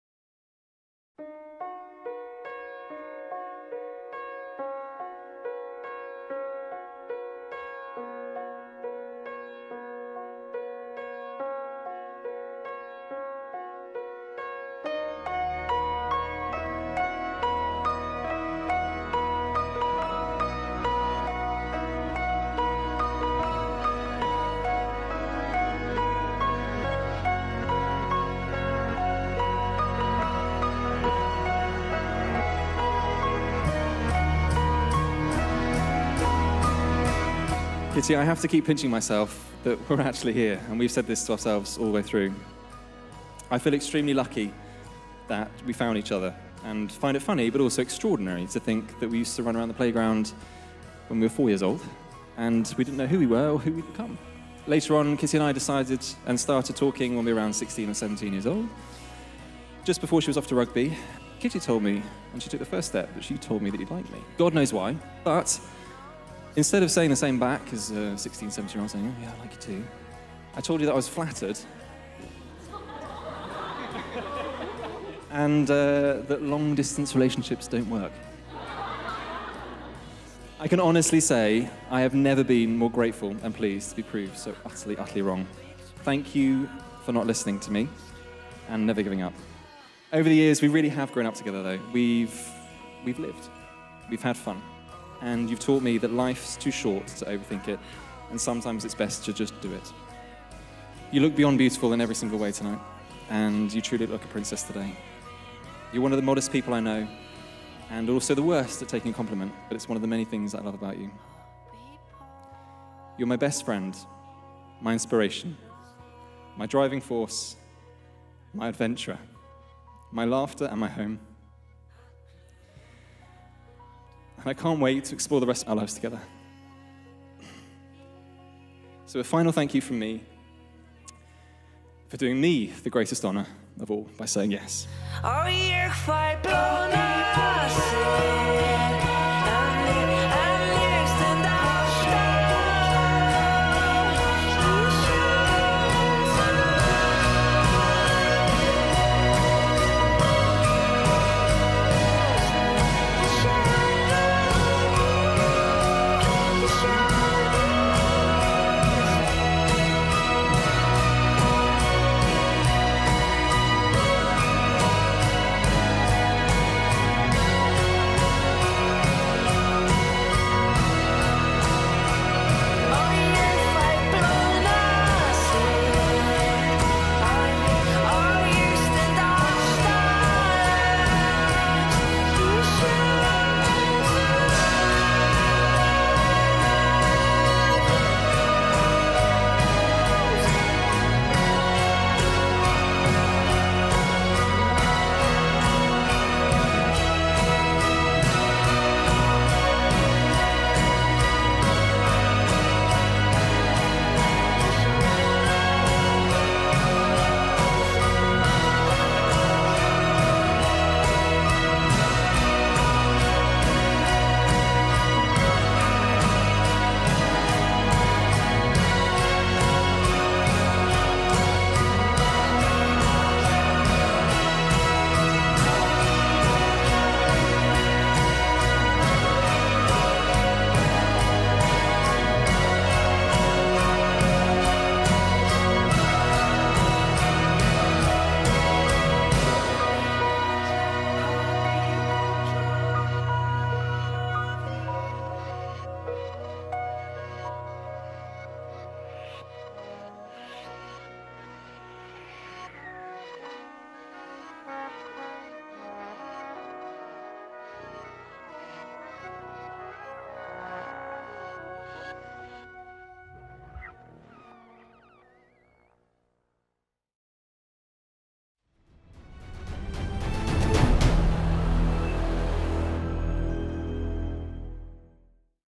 Autumn wedding at Royal Botanic Gardens, Kew
A perfect Autumn day for Kitty & Jamie's wedding at Kew Gardens. The marriage ceremony was held at St Anne's Parish Church on Kew Green and the wedding reception in the glorious Orangery, within the grounds of Kew Gardens.